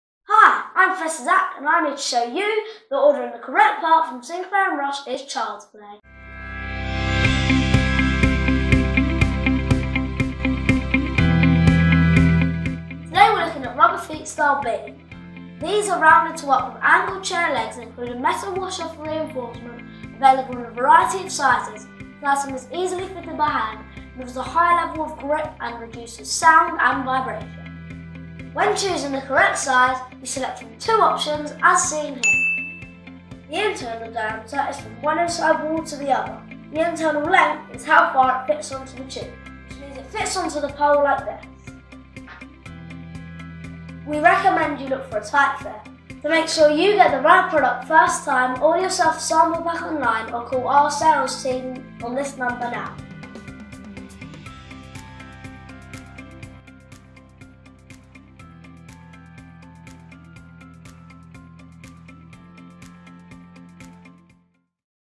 Rubber Feet Style B
Sinclair & Rush, manufacturer of Rubber, Plastic and Foam Component Parts introduces Professor Zac. Episode 2. Rubber Feet - Style B.

Product introduction and measuring guide. In this episode we concentrate of Style B Rubber Feet from our standard range of Plastic and Rubber Feet, available